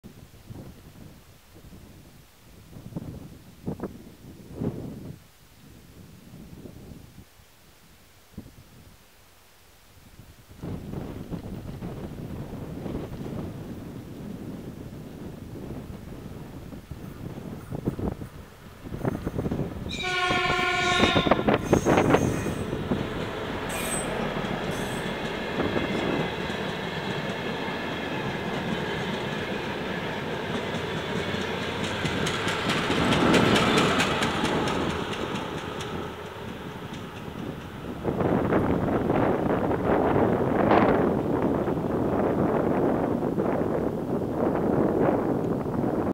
ДР1А-173 (БЧ, ТЧ-7 Брест) рейсом №6360 Брест - Лунинец
Перегон Жабинка - Кобрин (линия Брянск - Гомель - Брест, 770 км), Брестская область, Республика Беларусь, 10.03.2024, 16:58 (UTC+3).

DMU DR1A-173, Brest region, Republic of Belarus, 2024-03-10, 16:58 (UTC+3).

Поезд отправился с о.п. Батча, видного на заднем плане. Что стоит отметить, что тормозят дизель-поезда там быстро, а разгоняются весьма медленно: здесь уже более 1,2 км от старта, а скорость всего 45,2 км/ч.

Спасибо бригаде за приветствие и добросовестный труд, а БЧ - за благосклонность и доброжелательность к съёмкам!